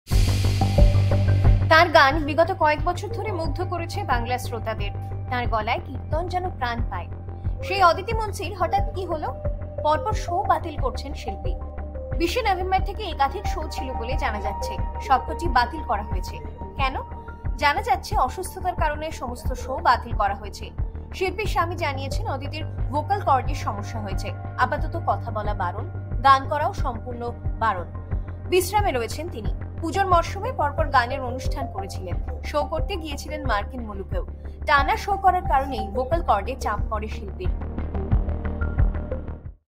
Aditi Munshi: পরপর শো বাতিল, আচমকাই অসুস্থ অদিতি মুন্সী, কী হয়েছে সঙ্গীত শিল্পীর?
২০ নভেম্বর থেকে তাঁর একাধিক শো ছিল বলে জানা যাচ্ছে, তবে সবকটিই বাতিল করতে হয়েছে। কেন? জানা জাচ্ছেন অসুস্থতার কারণেই সমস্ত শো বাতিল করা হয়েছে।